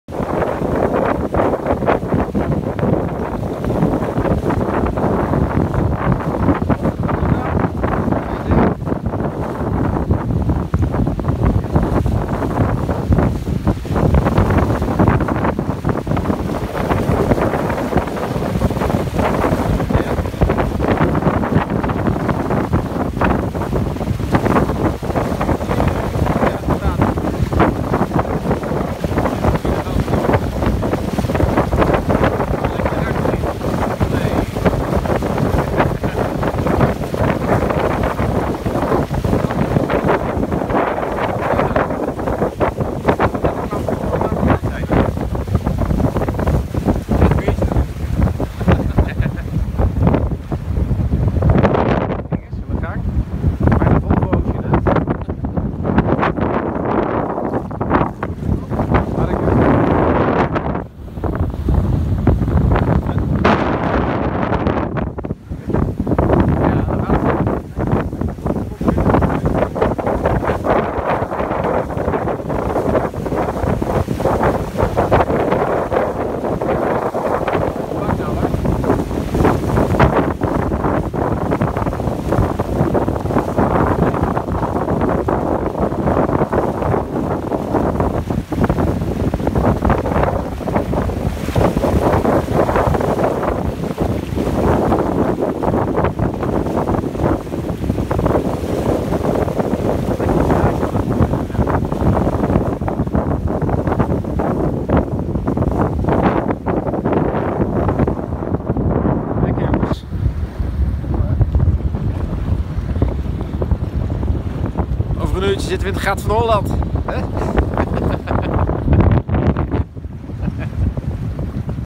Peer Gynt onder zeil
Zeilen Markermeer Peer Gynt mei 2018 met Ron